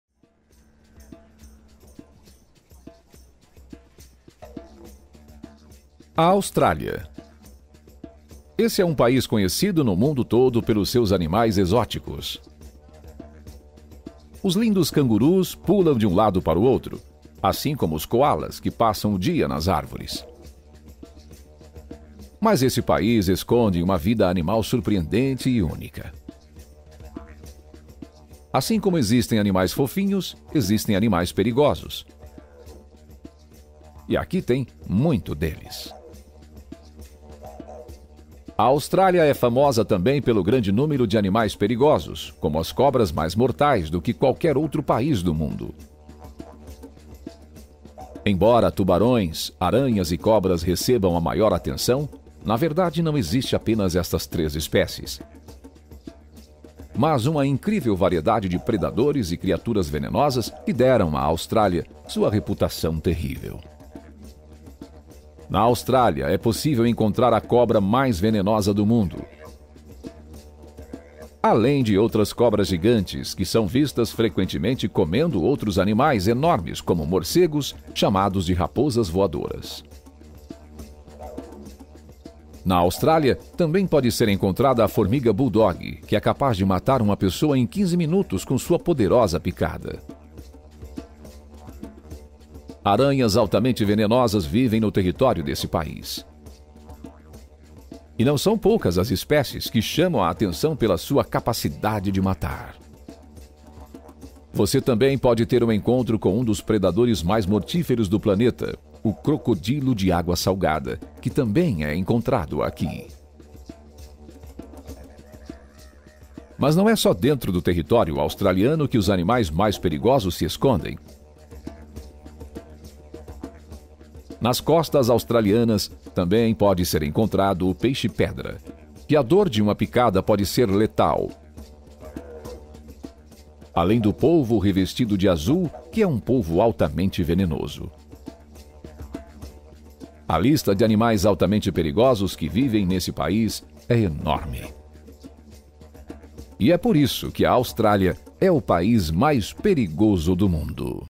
É POR ISSO QUE A AUSTRÁLIA É O PAÍS MAIS PERIGOSO DO MUNDO
Veja o motivo da Austrália ser o país maias perigoso do mundo